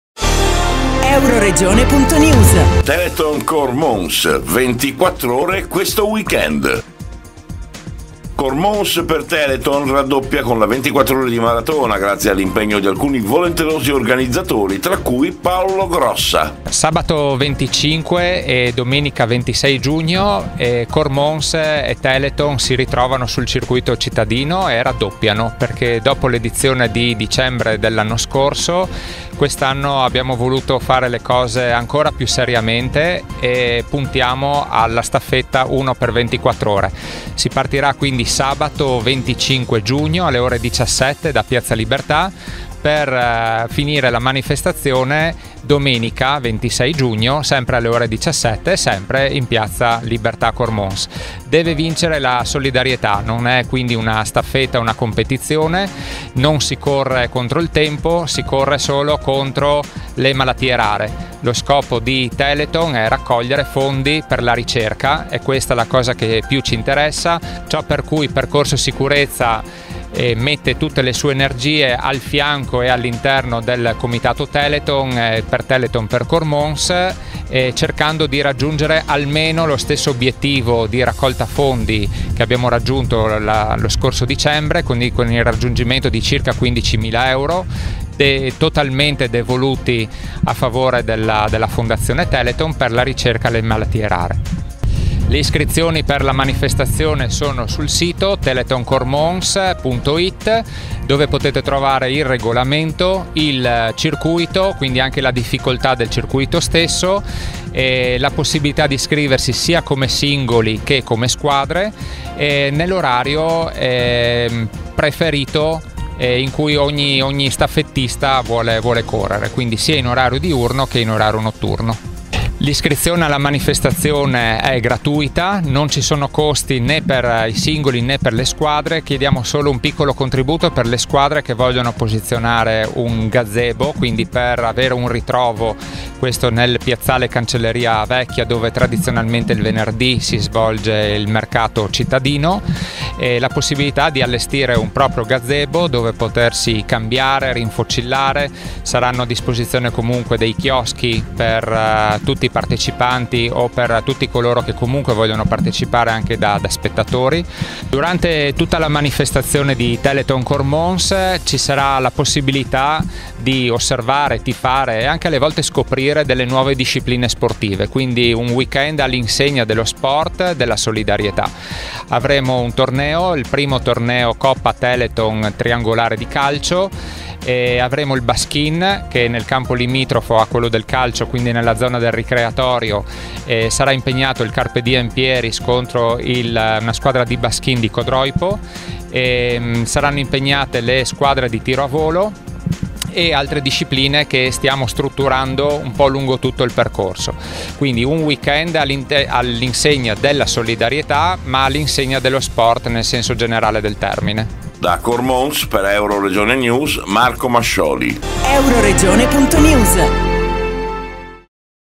TELETHON CORMONS – 24 ORE QUESTO WEEK END
Torna l’appuntamento sportivo di solidarietà a Cormons (GO), 24 ore per Telethon dalle 17 di sabato 25 giugno alle 17 di domenica 26. L’evento scaturito dopo quello dello scorso dicembre dato il successo della 12 ore che ha visto la partecipazione della cittadinanza e non solo, per la raccolta di fondi da devolvere in beneficenza.
Grande impegno per l’amministrazione comunale con il Sindaco Roberto Felcaro e dall’assessore allo sport Massimo Falato, ma soprattutto dalle aziende che partecipano attivamente e sponsorizzano l’evento.
Questa volta si raddoppia l’impegno con 24 ore di staffetta, ma con il coinvolgimento di altri sport ed eventi culturali. Il percorso è lo stesso circuito utilizzato in precedenza, sulle strade comunali di Cormòns, lungo un percorso cittadino ad anello di circa mt. 1000, da ripetersi ininterrottamente da ogni partecipante nell’ora prenotata all’iscrizione, nei giorni di sabato 25 o domenica 26 giugno 2022.
Le operazioni preliminari con...